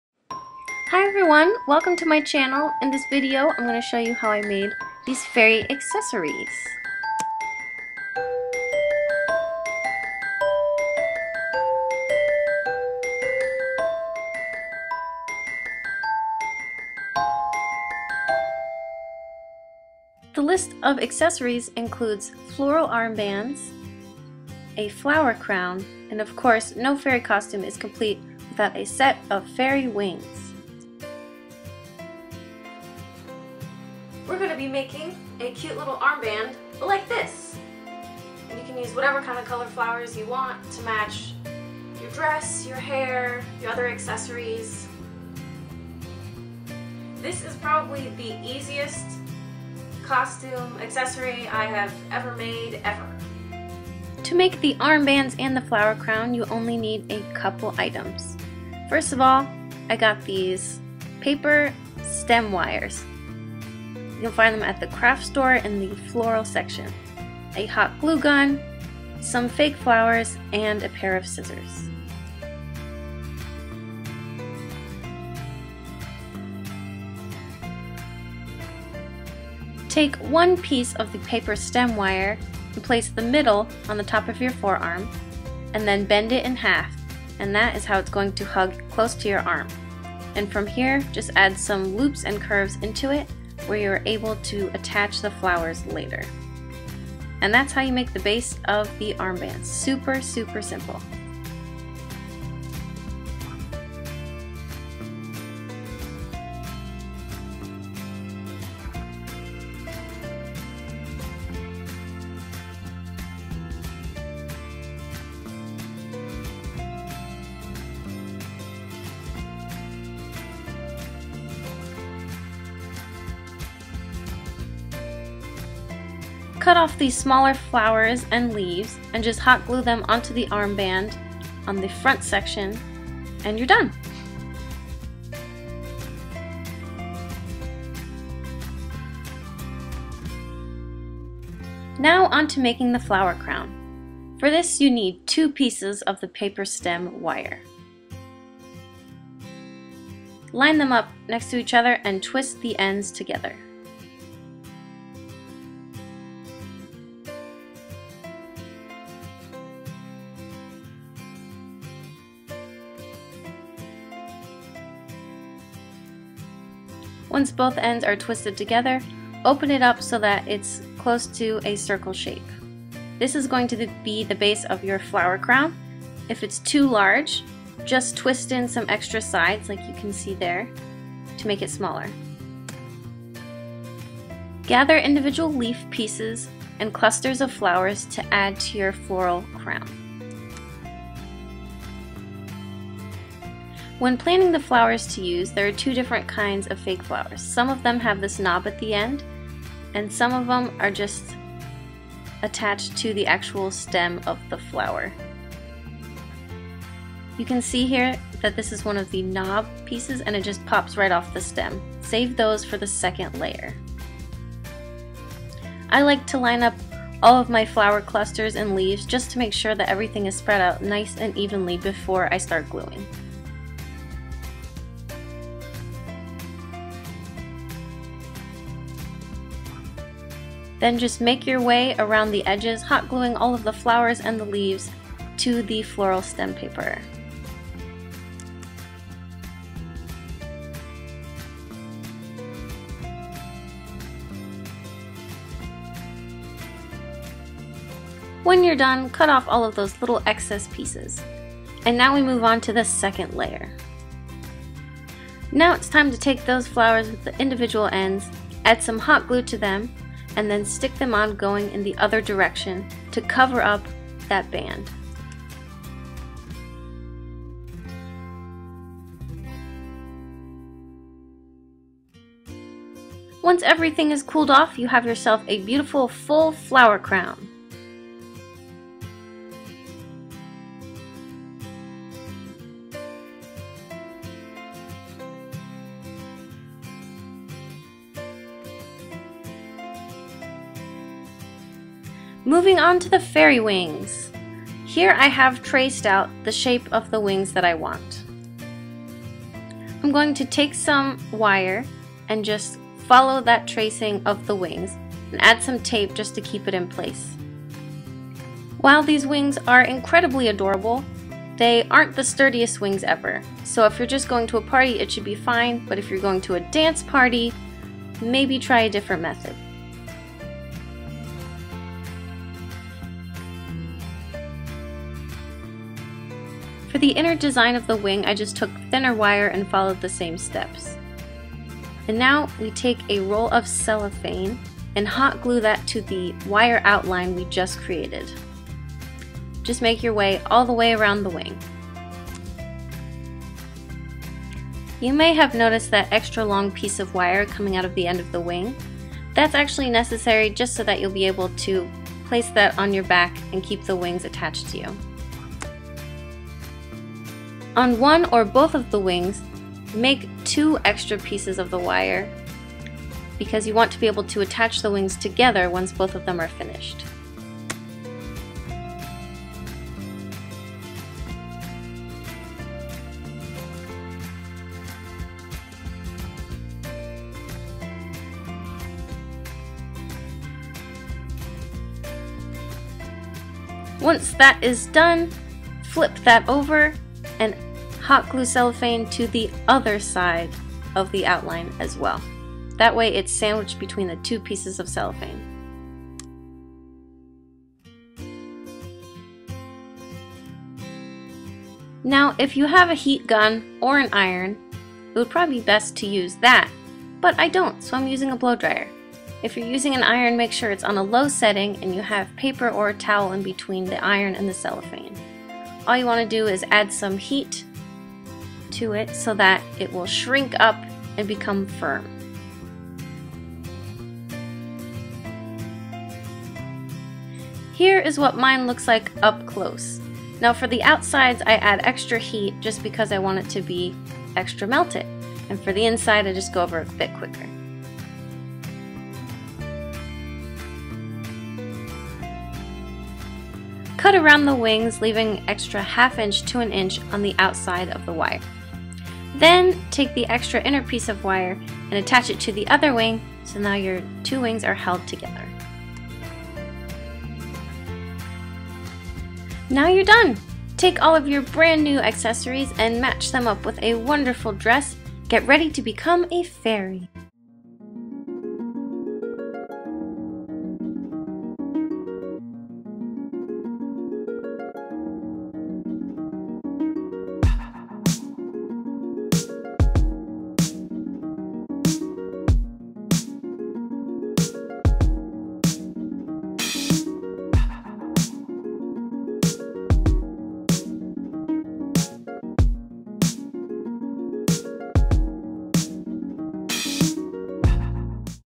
How To Make Fairy Accessories - Flower Crown, Floral Armbands, Wings
This is a tutorial on how to make fairy accessories, including flower armbands, a flower crown, and iridescent fairy wings. You can use these simple steps to create all kinds of different fairy looks by using different flowers, colors, and your own creativity.

Photography by Lydia Suarez at LydiaCooksGood